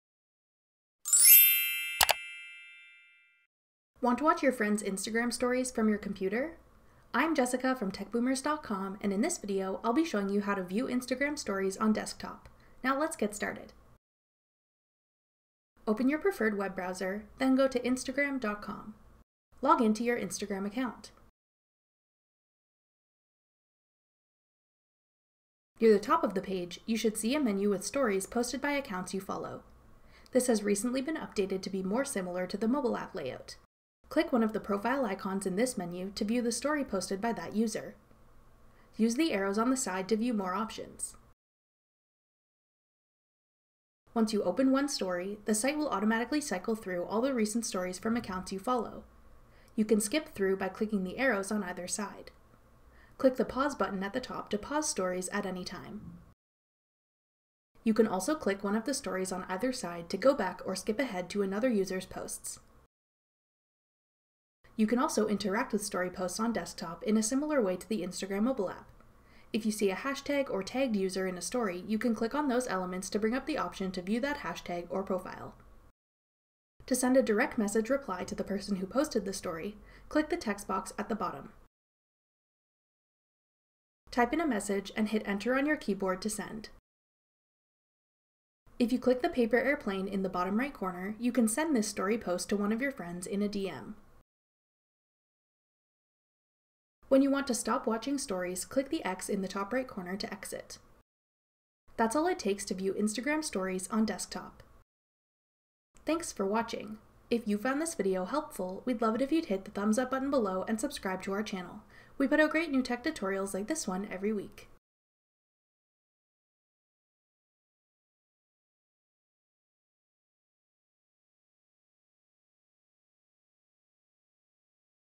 How to View Instagram Stories on Desktop
Want to view your friends’ Instagram stories from your computer? Watch this video to learn how to enjoy Instagram Stories on desktop.

Open your preferred web browser, then go to Instagram.com. Log into your Instagram account. Near the top of the page, you should see a menu with stories posted by accounts you follow. This has recently been updated to be more similar to the mobile app layout. Click one of the profile icons in this menu to view the story posted by that user. Use the arrows on the side to view more options. Once you open one story, the site will automatically cycle through all the recent stories from accounts you follow. You can skip through by clicking the arrows on either side. Click the pause button at the top to pause stories at any time. You can also click one of the stories on either side to go back or skip ahead to another user’s posts. You can also interact with story posts on desktop in a similar way to the Instagram mobile app. If you see a hashtag or tagged user in a story, you can click on those elements to bring up the option to view that hashtag or profile. To send a direct message reply to the person who posted the story, click the text box at the bottom. Hit enter on your keyboard to send. If you click the paper airplane in the bottom right corner, you can send this story post to one of your friends in a DM.  When you want to stop watching stories, click the X in the top right corner to exit.